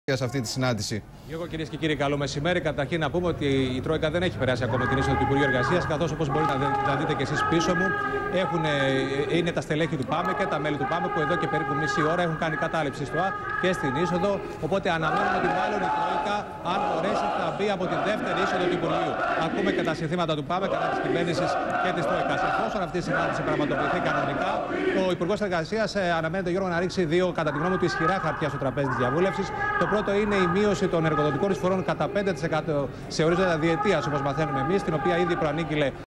Το ΠΑΜΕ απέκλεισε το Υπουργείο Εργασίας
Περίπου 50 άτομα, μέλη του ΠΑΜΕ έχουν προχωρήσει στον αποκλεισμό της εισόδου του υπουργείου Εργασίας.Η συμβολική κατάληψη γίνεται εν όψει της συνάντησης του υπουργού Γιάννη Βρούτση με τους επικεφαλής του κλιμακίου της τρόικας.